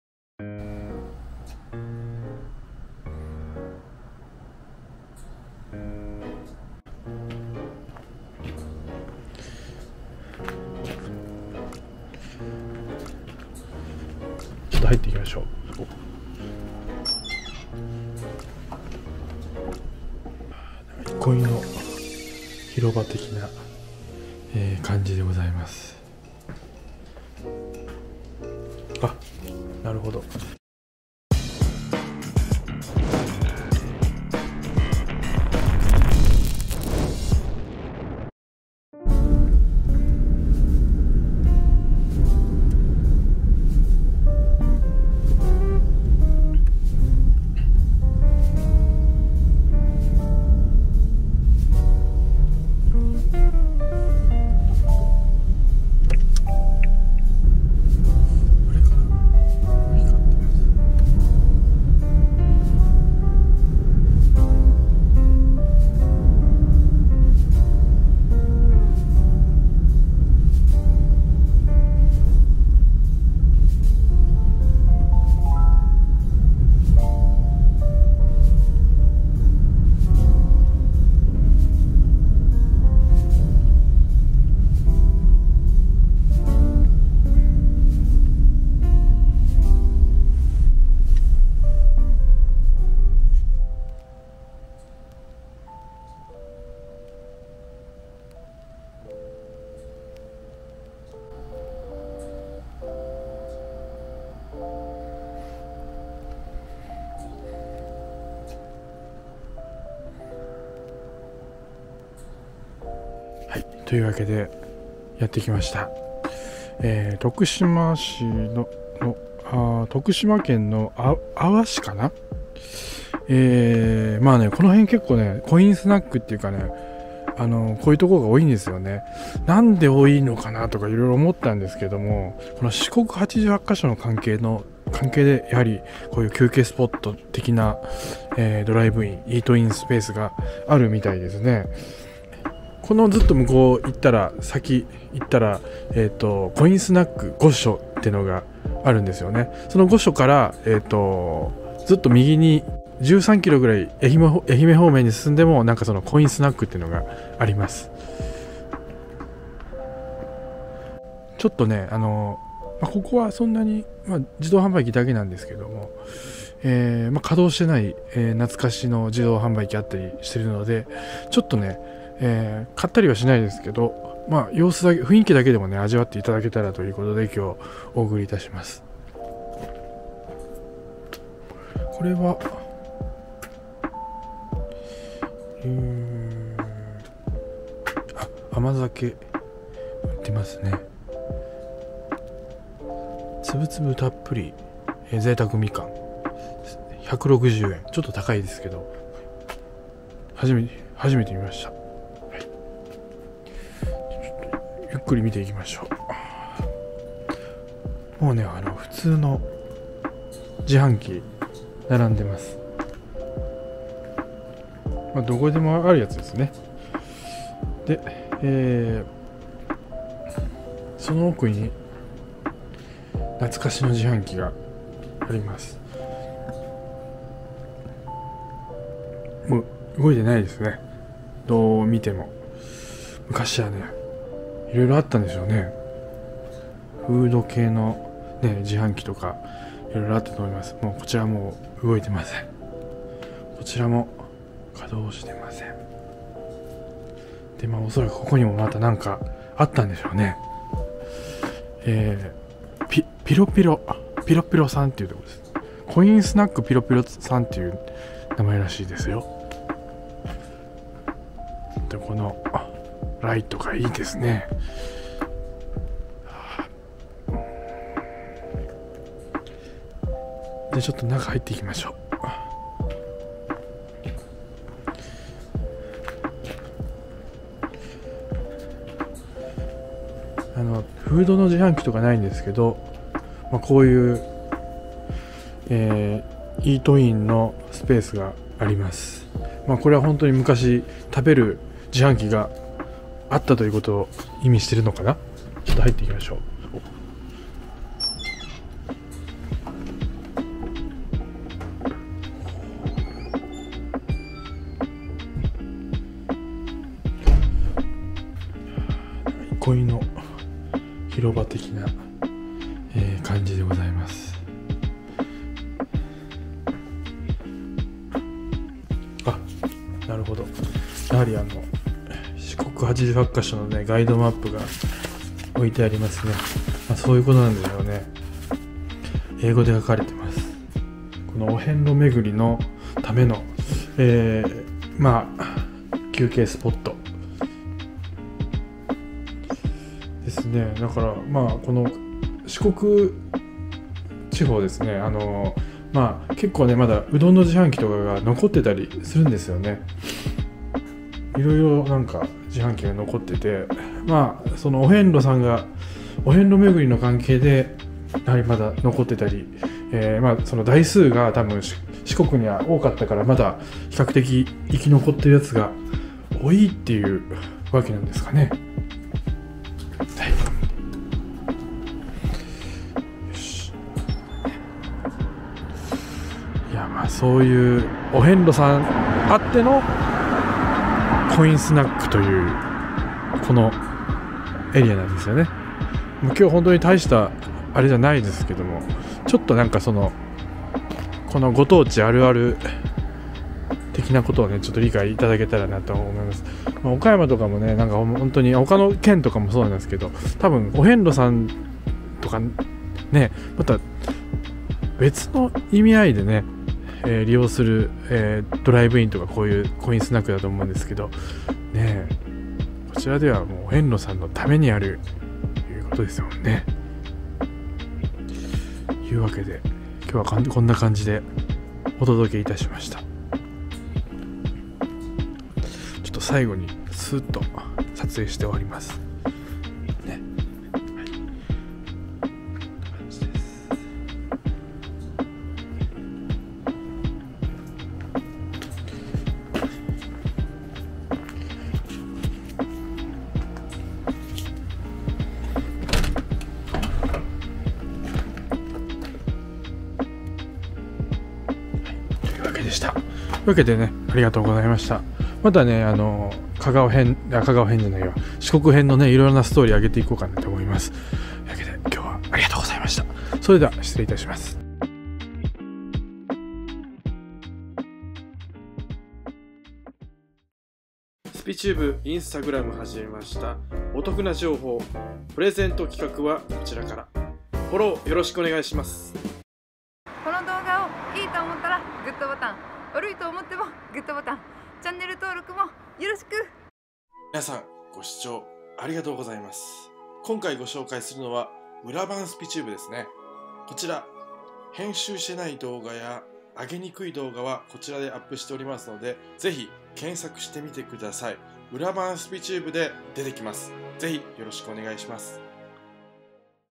徳島県阿波市【コインスナック ピロピロ】懐かしの自販機コーナー《お遍路さん休憩スポット》
昼間は休憩所横の小屋でたこ焼きが販売されているそうですので

お遍路さんや旅人が購入して休憩所で食べていたのかな。

ちなみに休憩所には週刊雑誌が山積みになっていたそうです。

 徳島県阿波市土成町高尾

再生リスト

これまでの番組協力など
RSKラジオ(土曜番長)

YOUTUBEコラボ
ちゃんけブログ